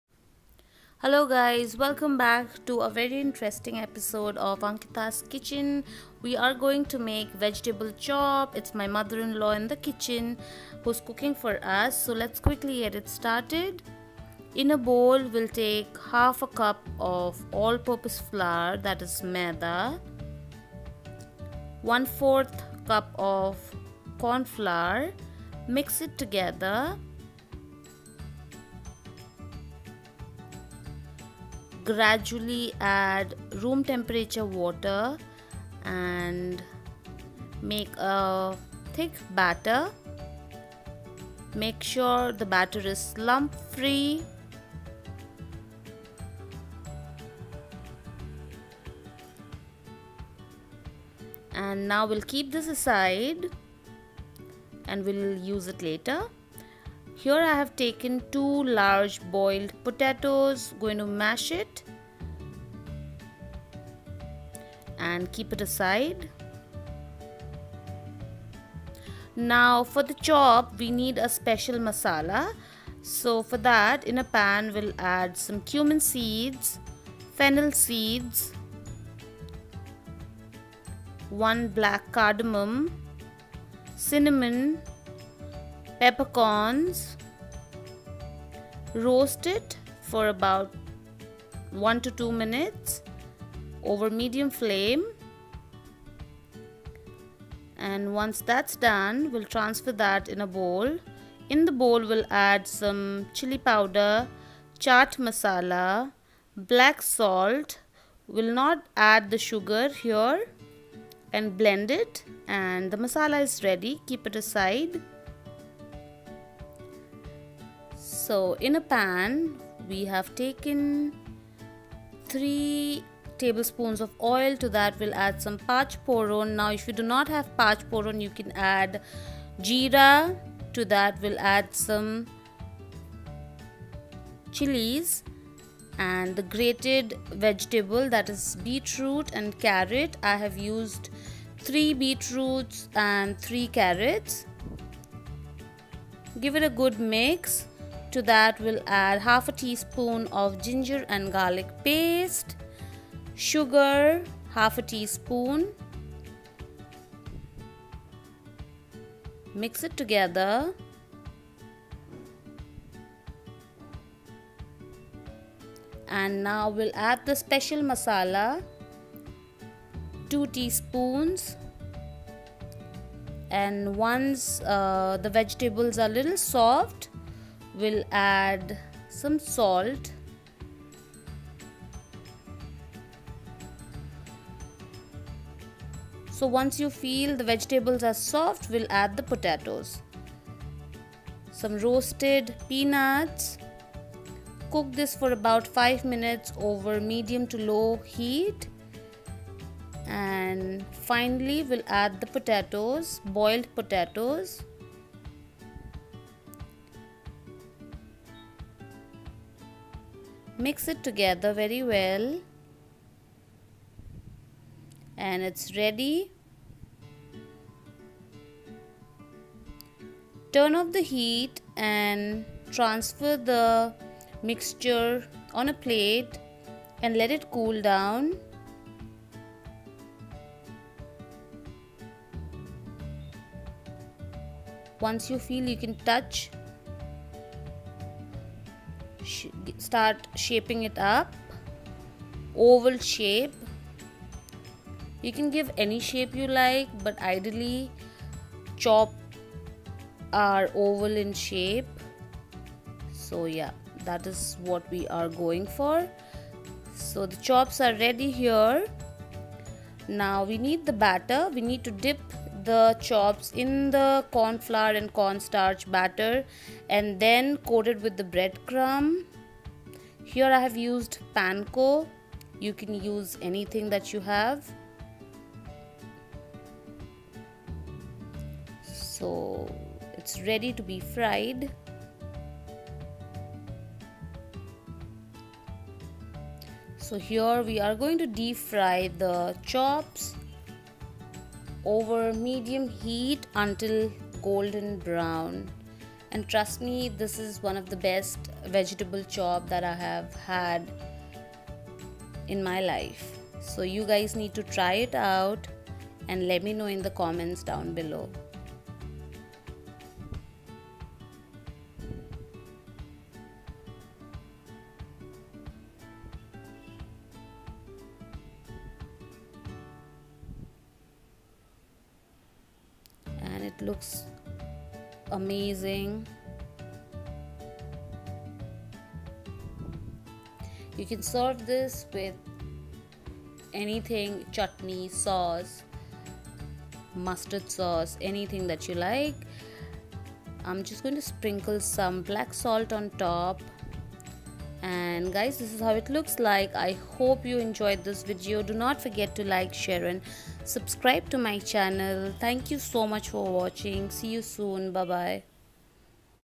Vegetable Chop| Kolkata Style| Bengali Chop| Snacks| Ankita’s Kitchen
Hi family,

Today, we’re going to make Kolkata Style Vegetable chop. Kolkata Style Vegetable Chop Recipe is a popular Kolkata Street food. These crunchy chops are made with beetroot, carrots and peanuts which gives a nice crunch. Make these for an evenings snack and enjoy with a hot cup of chai.
Vegetable chop is a popular street food from Kolkata. Beetroot,carrot, peanuts and other vegetables are cooked and coated with breadcrumbs then deep fried till golden brown and served with kasundi (A mustard based sauce) and onion cucumber slices. Being loaded with vegetables it's great after school snack for kids.  Cutlets recipes are very common across india and are generally prepared with either combination of veggies or meat. The bengali cuisine has its own variation and it is referred as vegetable chop recipe. It is prepared with a combination of veggies, but beetroot has significant importance.

Loads of love and happiness.